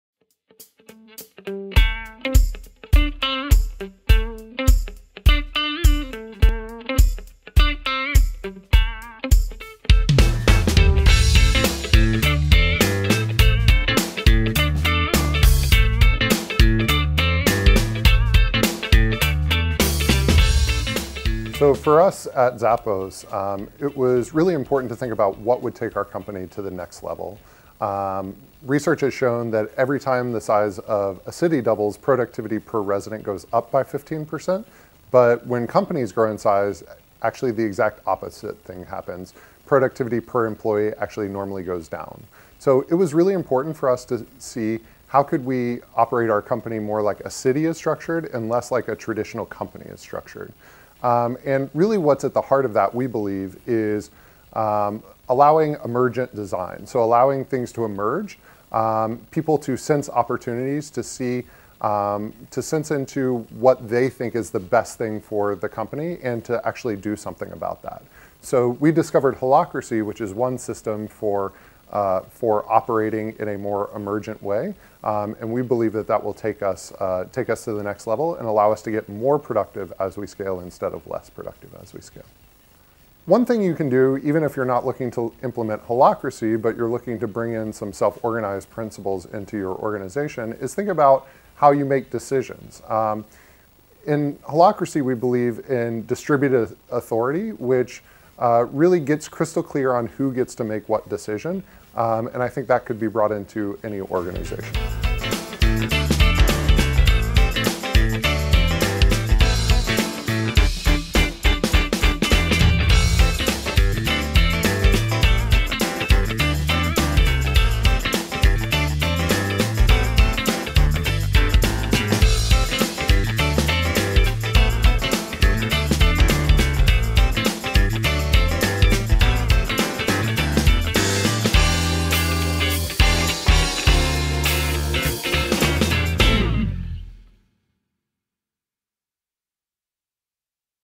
How to take your company to the next level - experience at zappos - John Bunch
John Bunch about Holacracy at Zappos
and how to take your company to the next level

recorded at next:organizing
a future lab for managers who want to know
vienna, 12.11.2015

hosted by
hernstein institut
dwarfs and Giants
evolution at work
beratergruppe neuwaldegg